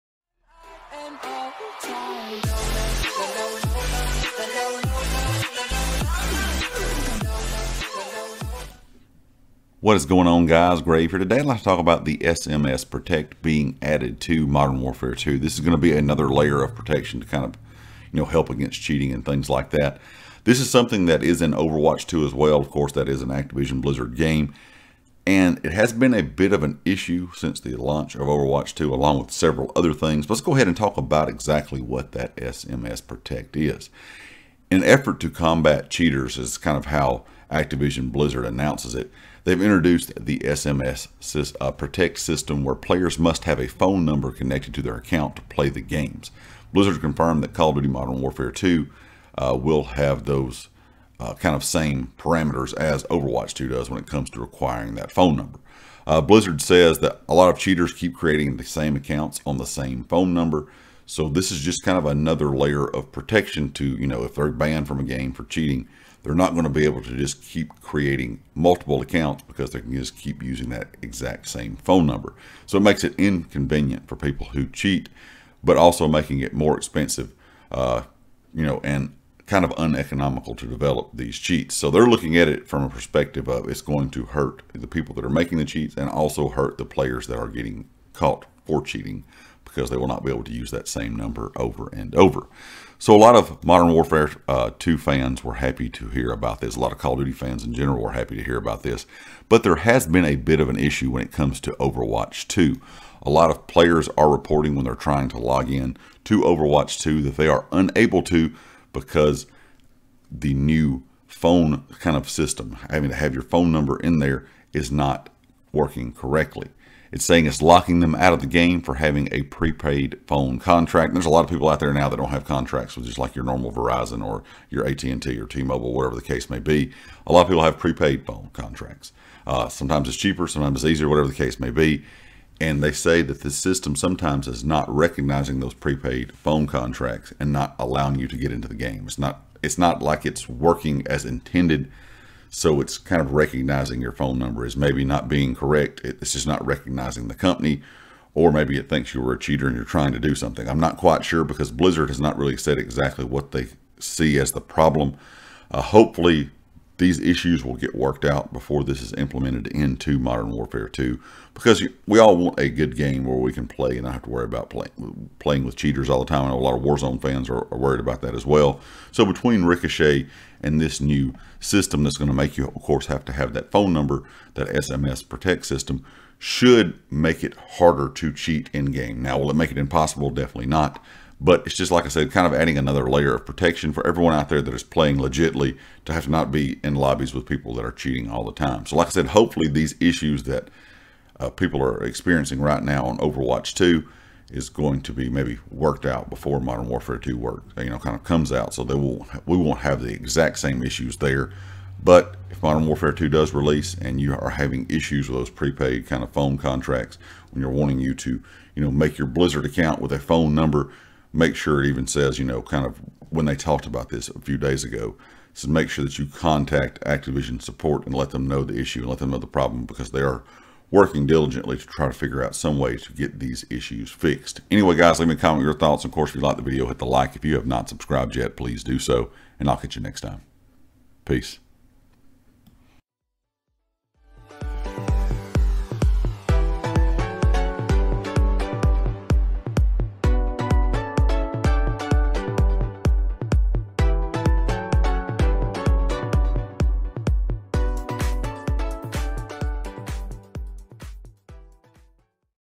😍SMS Protect System Coming To MW2
Modern Warfrare will have the SMS protcet system to help with cheaters making more accounts.